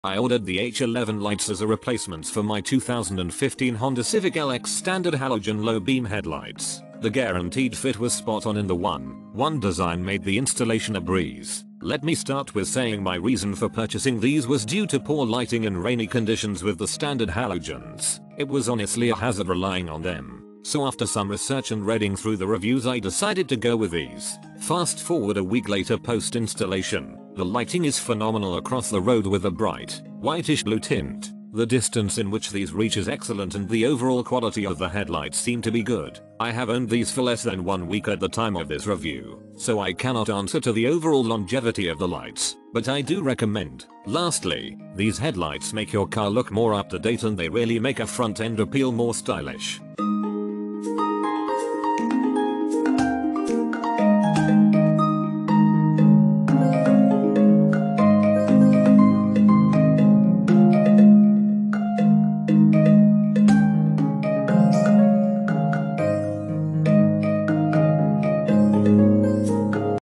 SEALIGHT Scoparc S2 H4//9003//HB2 LED Headlight Bulbs, 9003 LED High Beam Low Beam, review
Main Features:
by entering your model number.. 5 Minutes Easy Installation: 1:1 in Size with a Halogen Bulb, no external driver or connector. Perfectly fit into your housing and factory sockets, and even in tight spaces. 1200 Ft Visibility: Ditch your dim yellow halogens and make way for 6500K bright white headlights. Super focused beam pattern design provides wider and farther lighting range, adding confidence in night driving. Say Goodbye Halogen: Aluminum construction and turbofan ensure a longer lifespan of up to 50000 hours, which equals 100 halogen bulbs. No more frequent replacements. Provide 60% Cooling: 12000RPM powerful turbofan provides up to 60% better cooling compared to those without fans. Just like drying hair with a hairdryer, which is more efficient than to dry it naturally. 99% vehicle adaptability: S2 H11 bulb features superior CSP LED chips, producing 15000lm light output per set. Compatible with 99% of vehicles without error warnings or radio interference